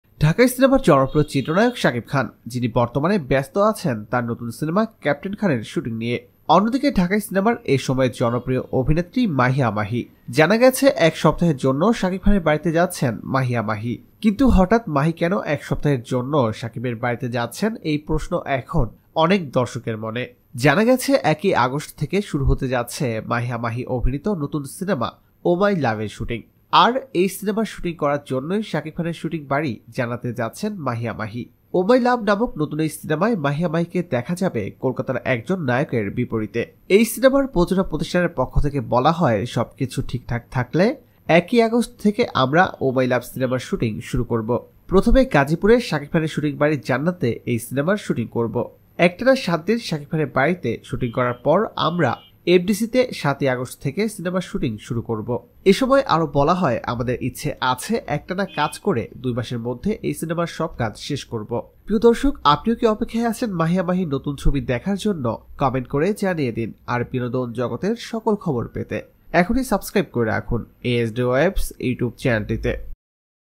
শাকিব খানের ঘরে যাচ্ছেন মাহিয়া মাহি | Mahiya Mahi with Shakib Khan
ঢাকাই সিনেমার জনপ্রিয় চিত্র নায়ক শাকিব খান। যিনি বর্তমানে বেস্ত আছেন তার নতুন সিনেমা ক্যাপ্টেন খানের শুটিং নিয়ে। অন্যদিকে ঢাকাই সিনেমার এ সময়ের জনপ্রিয় অভিনেত্রী মাহিয়া মাহি। জানা গেছে এক সপ্তাহের জন্য শাকিব খানের বাড়িতে যাচ্ছেন মাহিয়া মাহি। শাকিব খানের ঘরে যাচ্ছেন মাহিয়া মাহি | Mahiya Mahi with Shakib Khan | Latest Bangla News